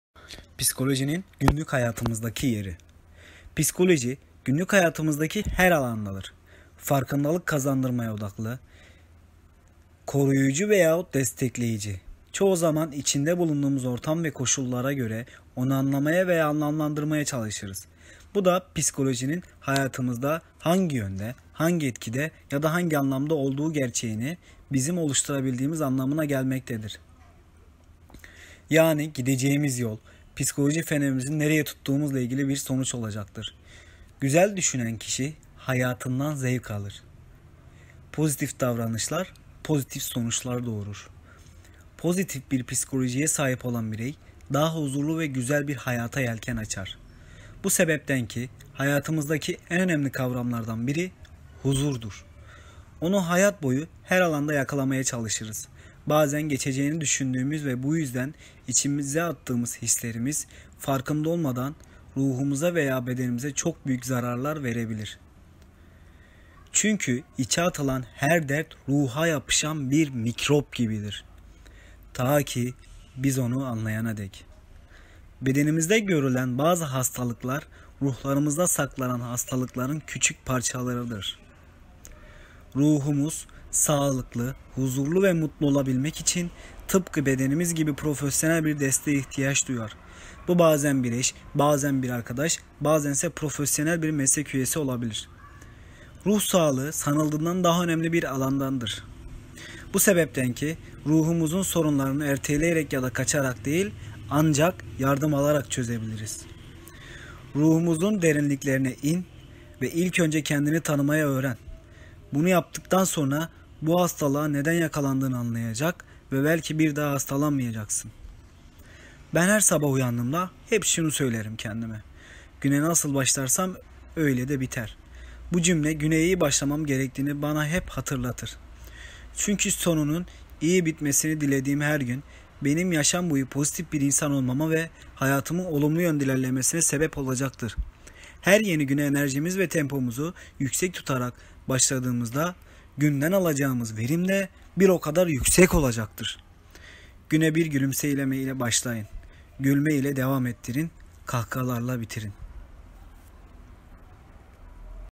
Psikolojinin Günlük Hayatımızdaki Yeri
Sağlık Asistanı Platformu yazarı Çağdaş Çuhadar, Sağlık Asistanı Dergisi için yazmış olduğu yazıyı sizler için seslendirdi.